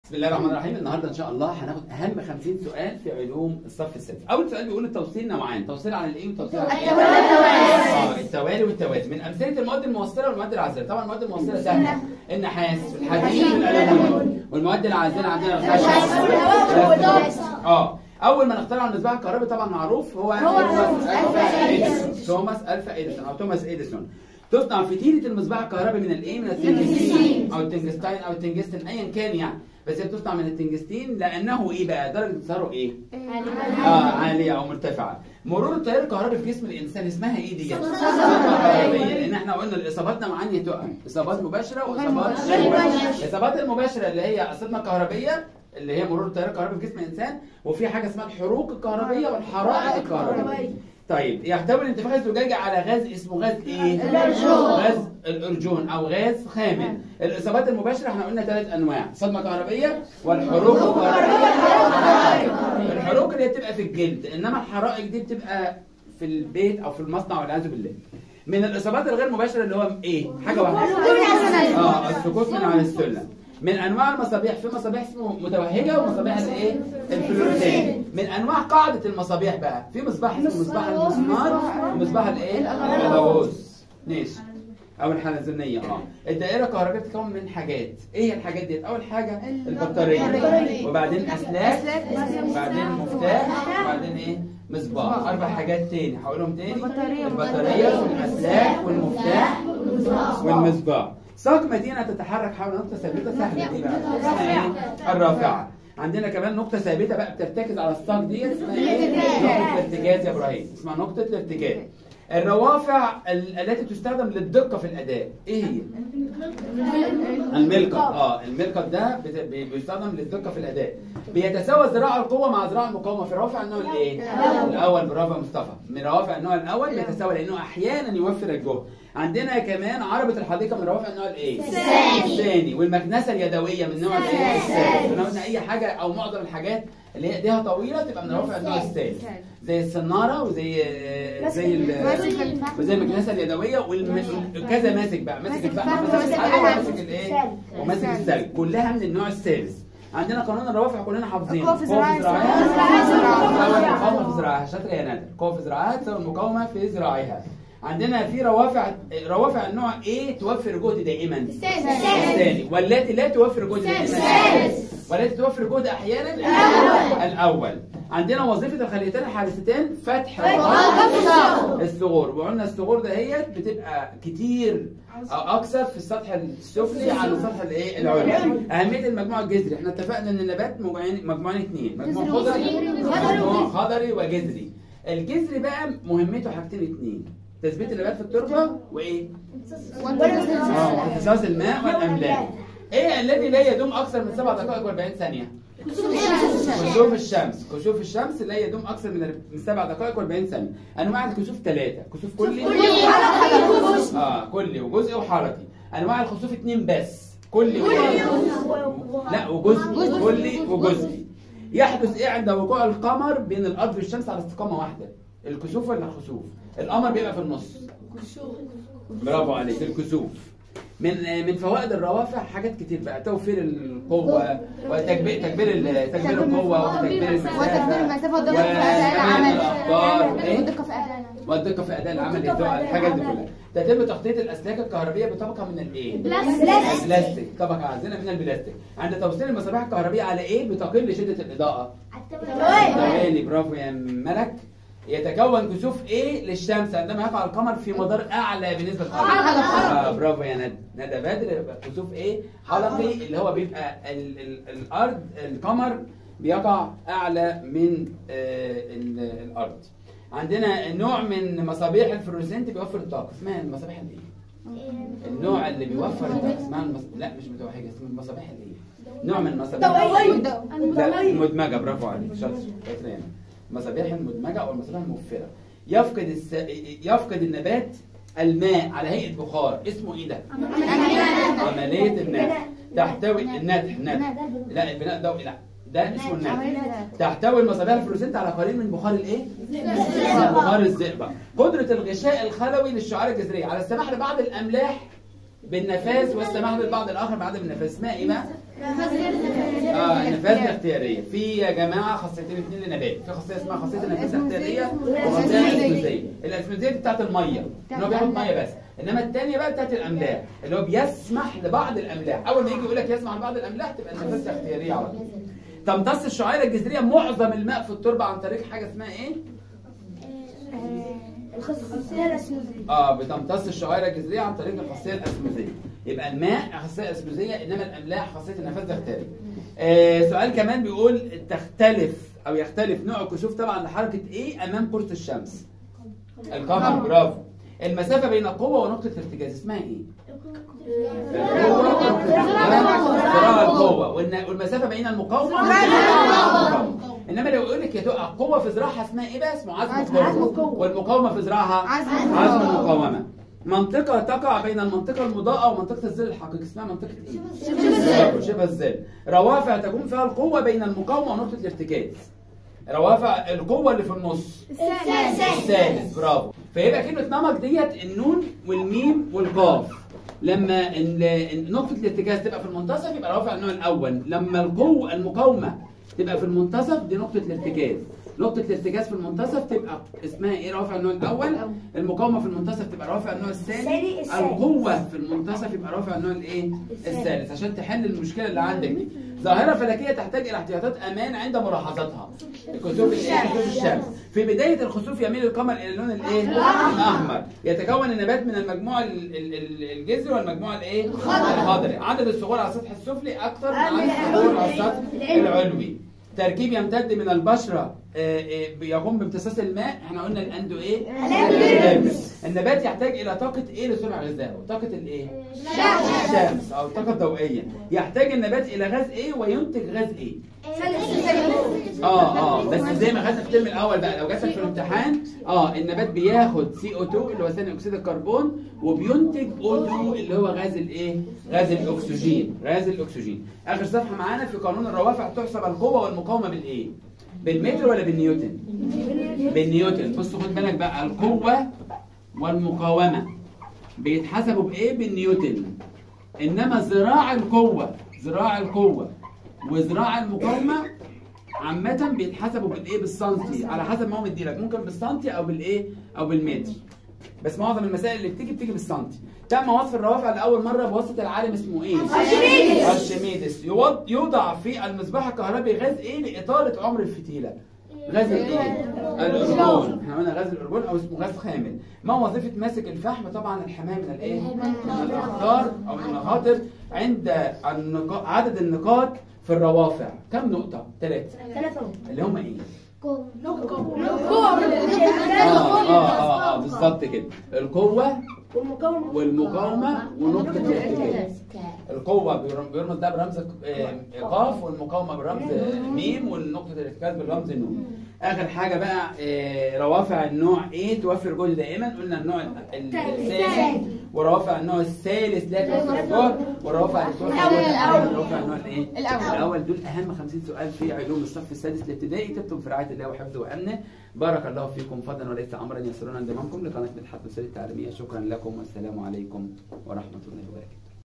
أهم 50سؤال في علوم الصف السادس الابتدائي الترم الثاني
مراجعة علوم الصف السادس الابتدائي الفصل الدراسي الثاني أهم50سؤال